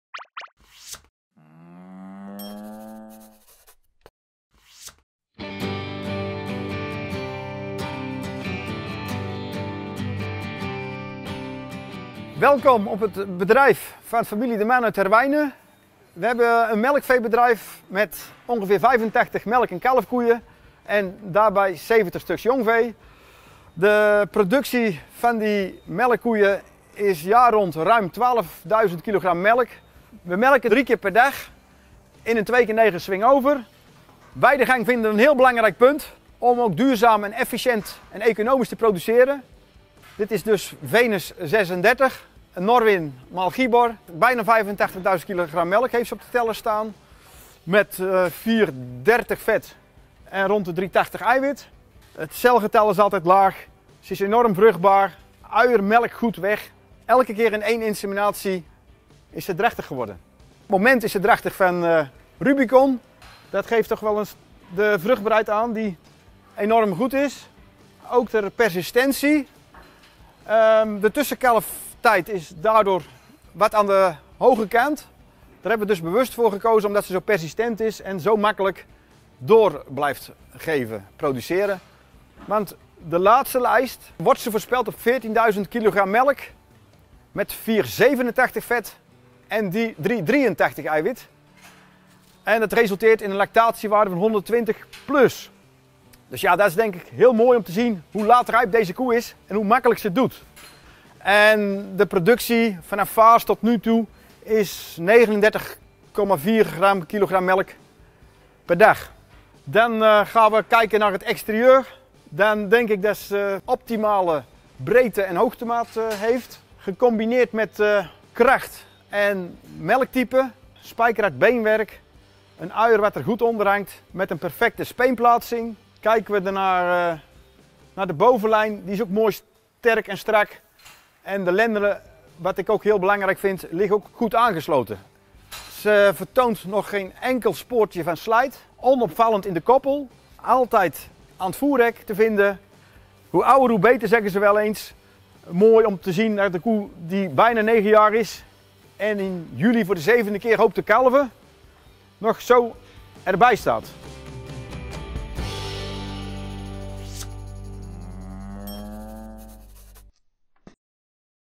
Venus 36 van de familie De Man uit Herwijnen (GD) - cat. jong - Beste Boerenkoe 2022
Venus 36 (Norwin x Gibor) van de familie De Man uit Herwijnen (GD) is met haar 8 jaar en 8
 maanden de oudste deelnemer in de categorie Jong. De jury omschrijft Venus 36 als een fraaie
 koe met goede verhoudingen. „De koe is goed ter been, heeft voldoende diepte in de voorhand
 en haar zeer fraaie frame heeft geweldig mooie overgangen. Daarbij vertoont Venus 36
totaal geen sporen van slijtage”, aldus de jury. Met één inseminatie per lactatie valt op haar
 vruchtbaarheid niets af te dingen. En ook de uiergezondheid is dik in orde met één celgetalverhoging
 in zes lactaties. Venus 36 is een plezierige koe met een attent karakter, aldus Jan de
 Man. „Ze ziet er nog erg jeugdig uit, heeft een goed bewaard uier en harde benen.” Met 39,11
kilo melk per productieve levensdag behoort de koe tot de koplopers in haar categorie. Ook
 met de kilo’s vet en eiwit per productieve levensdag scoort ze bovengemiddeld.